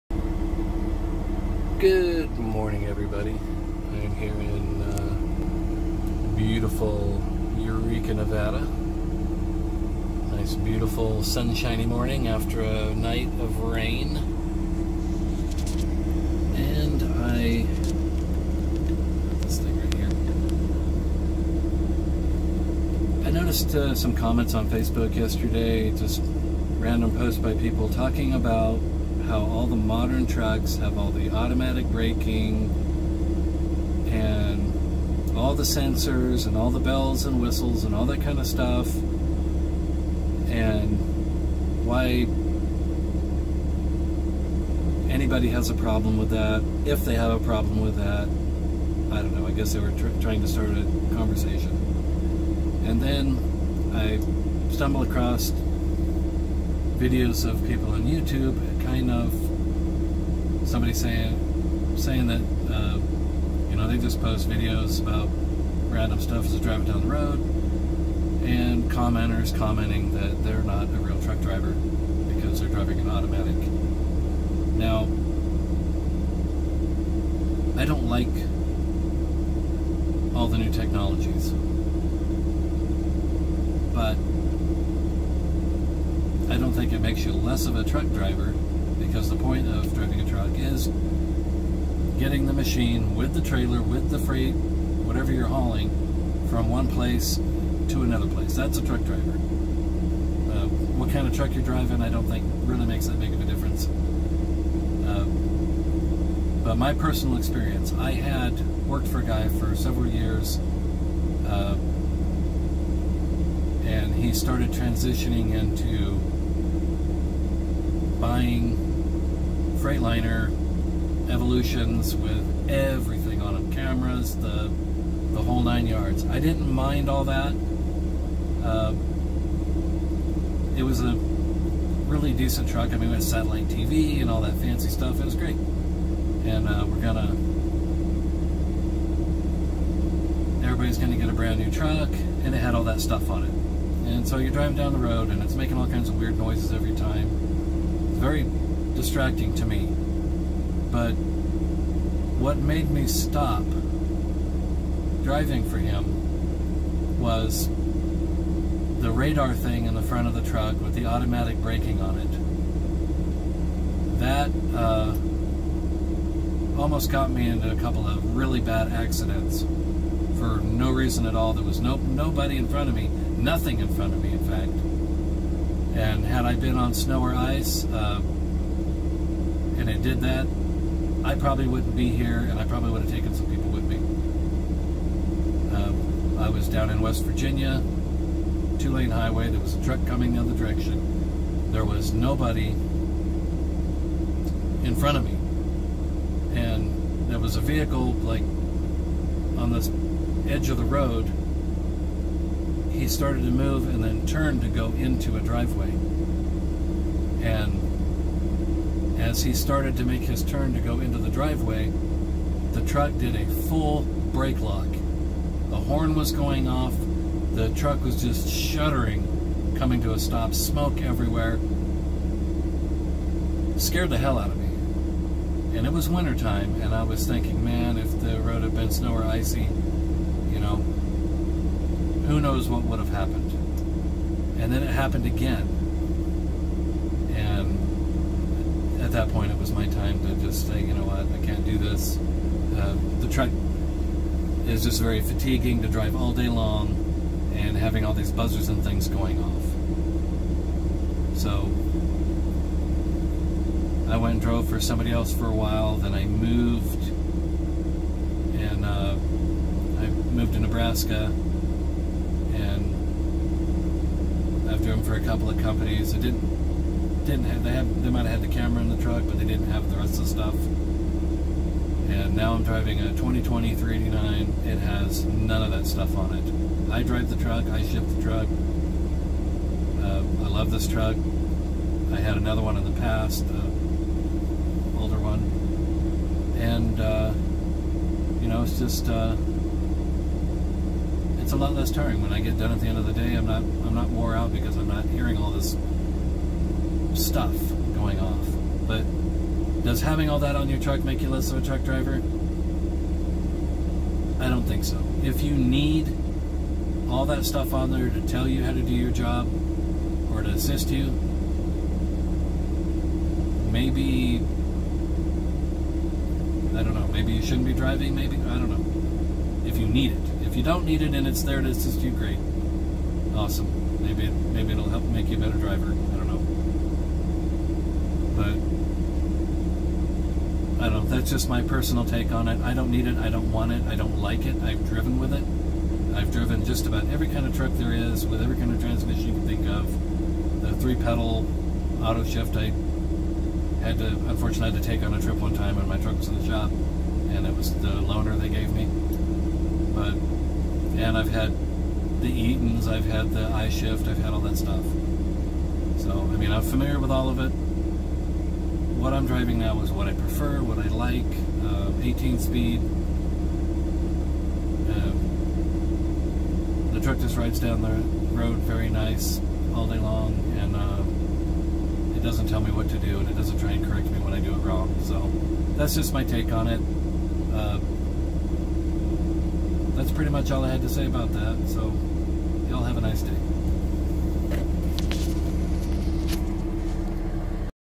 Auto Braking nearly killed me
My take on the new technology in these trucks.  And my personal experiences driving them.